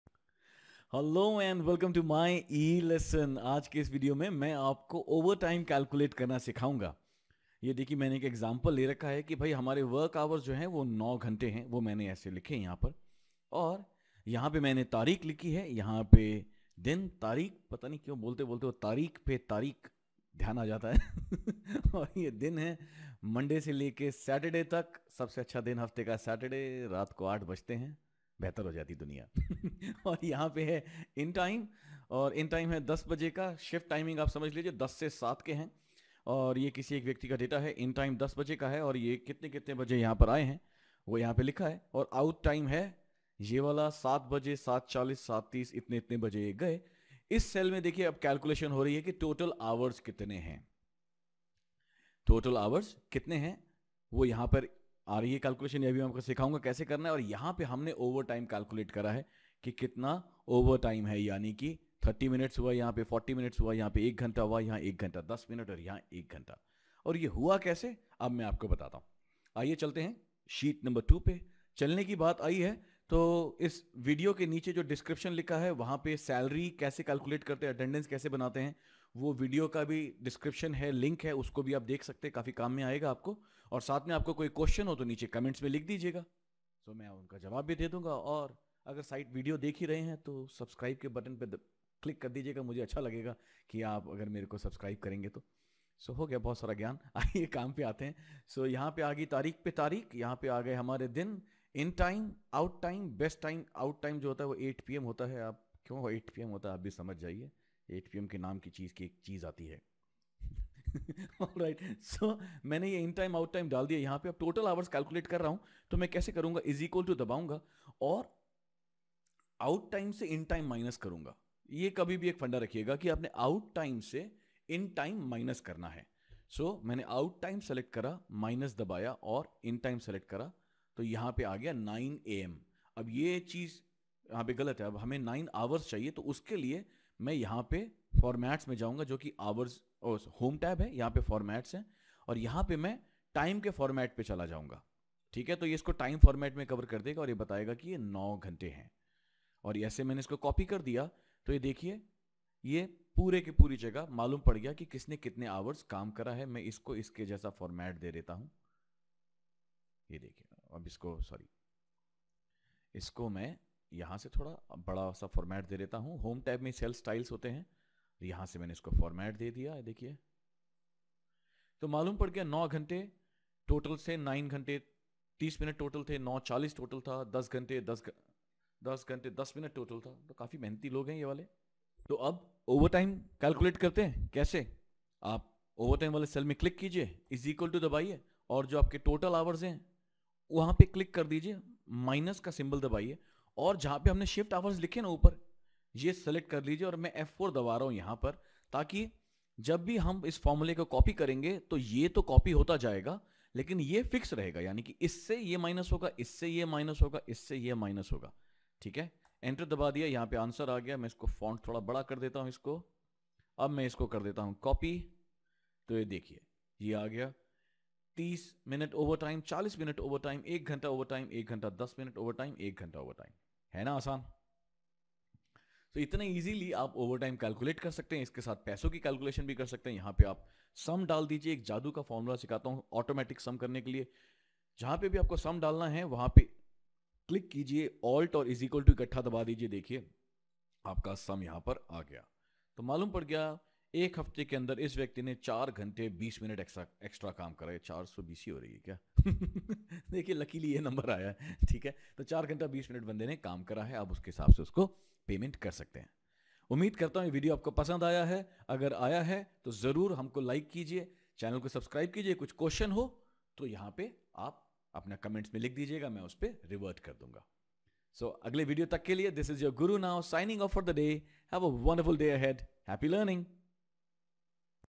Calculate Overtime in Excel Hindi ☑️
Learn How to calculate overtime in Excel in 3 minutes ! Many times need to find out extra time that a worker has spent at work and its actually very easy to find out the Employee Overtime in Excel. To calculate the Overtime in Excel I have used 3 things
1- Sum Formula
2-  Minus Function
3 - Time Format
  It’s a very easy way to find the overtime of any worker in your office.